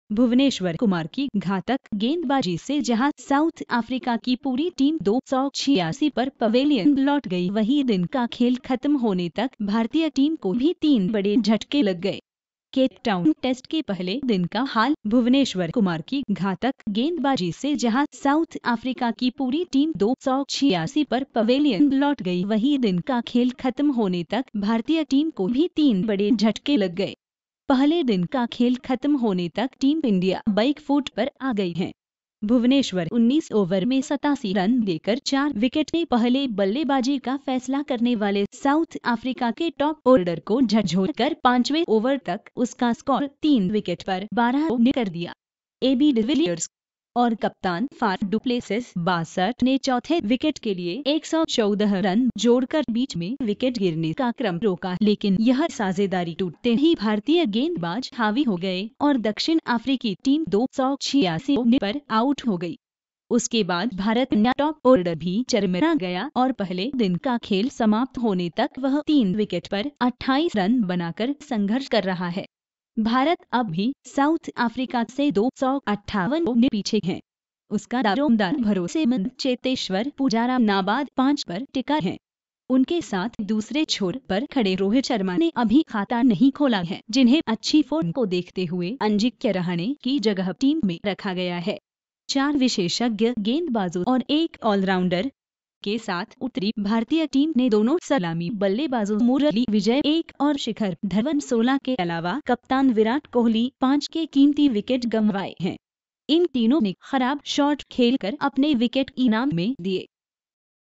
India vs South Africa 1St test - Day 1 Full Match Highlight 1st Test Cape Town, Jan 5-9 2018
India vs South Africa 1St test - Day 1 Match Full Match Highlight 1st Test  Cape Town, Jan 5-9 2018